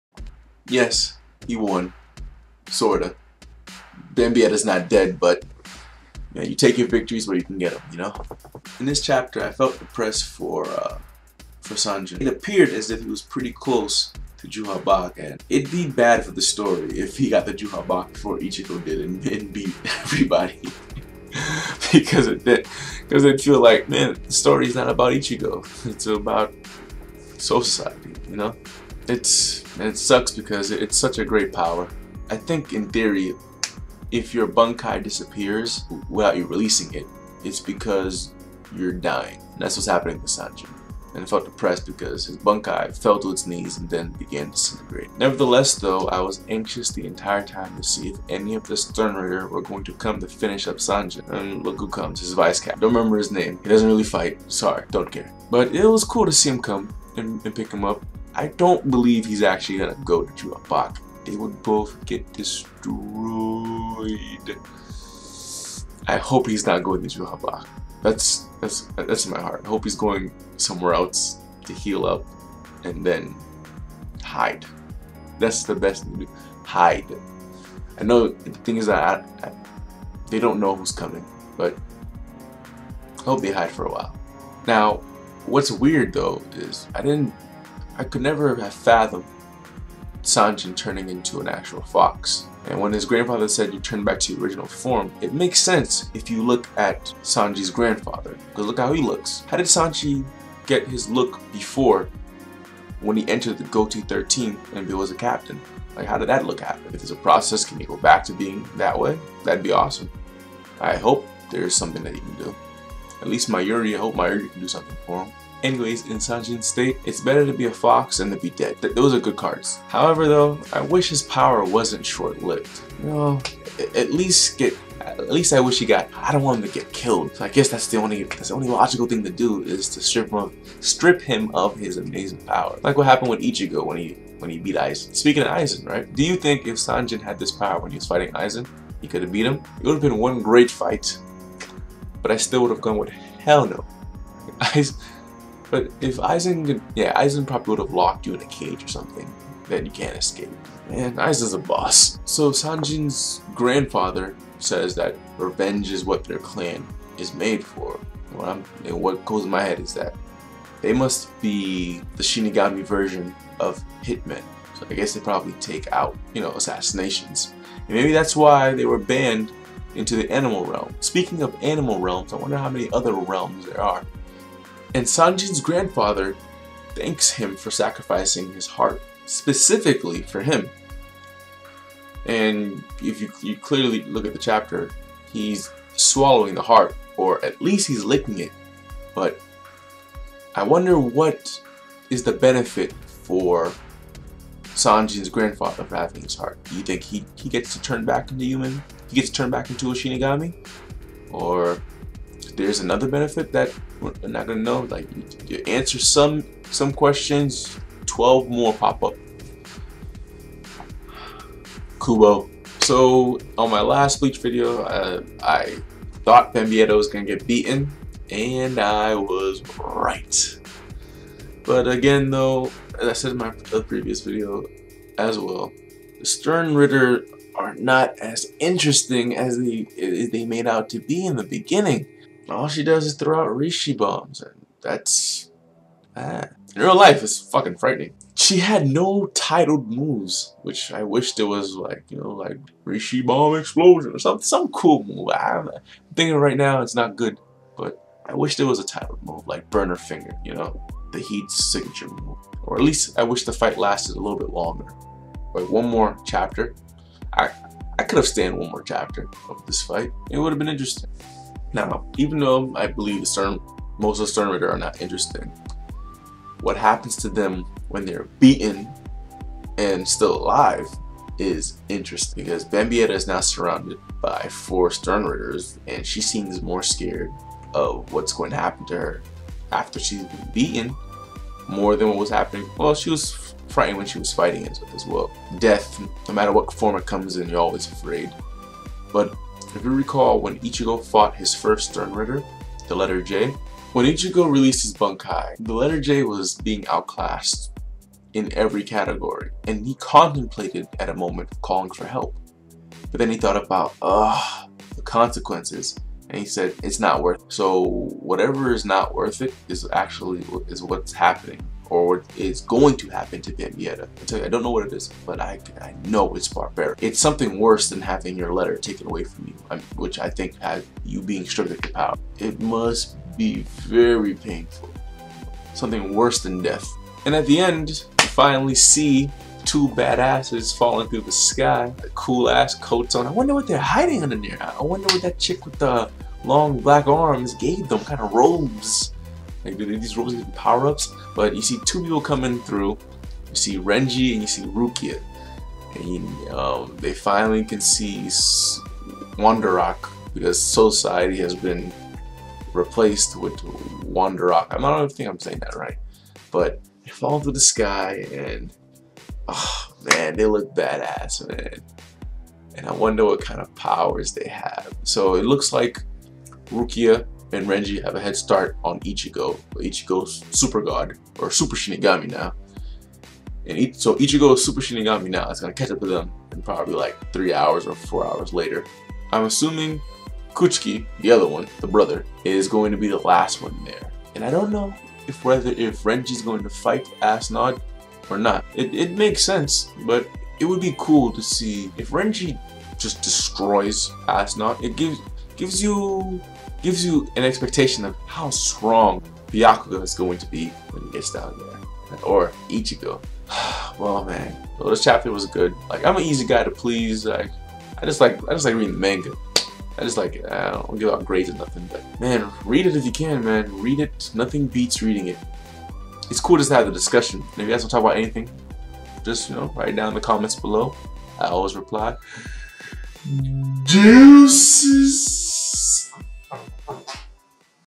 Bleach 558 The Heart Of The Wolf
Will Sajin Komamura live on? What about Bambietta?